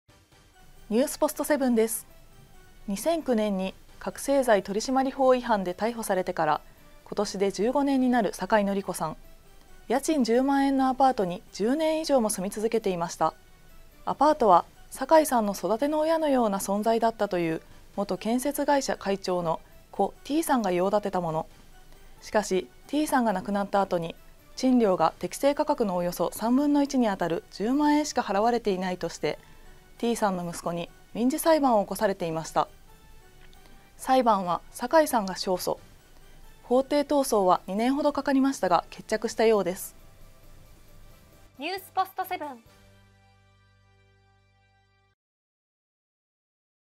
酒井法子 「 家賃 10万円アパート」に暮らし続ける 裏事情 NEWSポストセブン
2009年に覚せい剤取締法違反で逮捕されてから、今年で15年になる酒井法子さん。家賃10万円のアパートに10年以上も住み続けていました。

アパートは、酒井さんの育ての親のような存在だったという元建設会社会長の故・Tさんが用立てたもの。しかし、Tさんが亡くなった後に、賃料が適正価格の約3分の1にあたる10万円しか払われていないとしてTさんの息子に民事裁判を起こされていました。

裁判は、酒井さんが勝訴。法廷闘争は2年ほどかかりましたが、決着したようです。

◆酒井法子「家賃10万円アパート」に暮らし続ける裏事情 NEWSポストセブン

アナウンサー：福永桃子